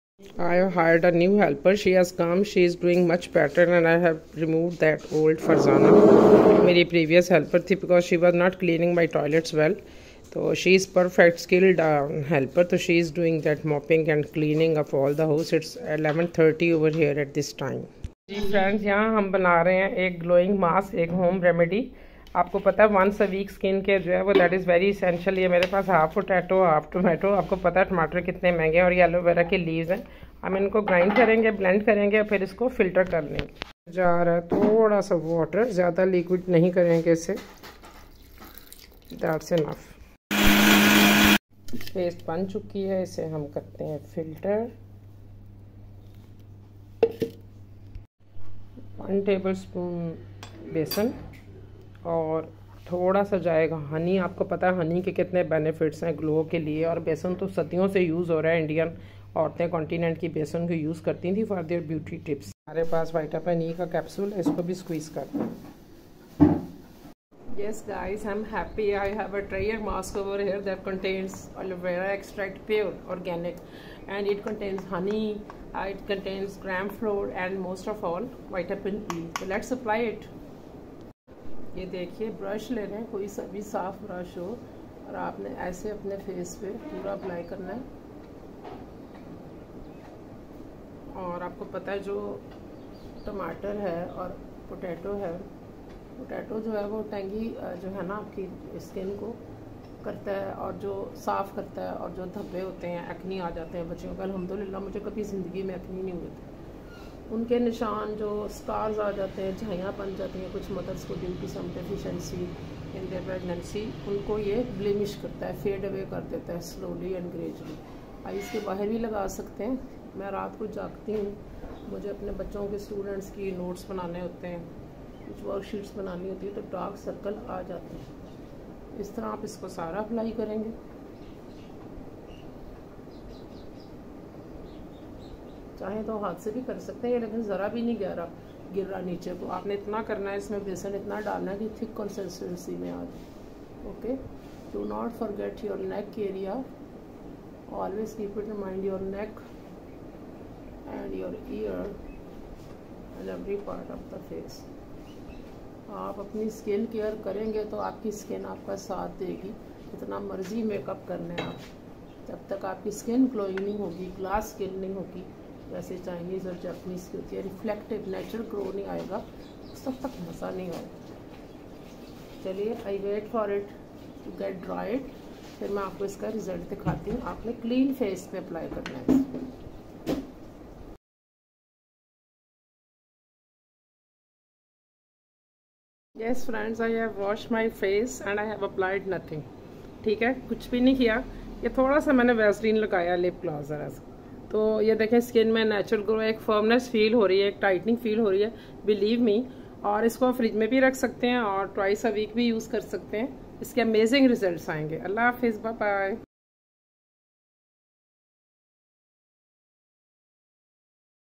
glowing skin mask | home made skin brightening mask | home cleaning | morning vlog
dear friends ,This is today's vlog when I managed to hire my new helper today and got some time to make an amazing mask using tomato, aloevera and potato .it turned out so good.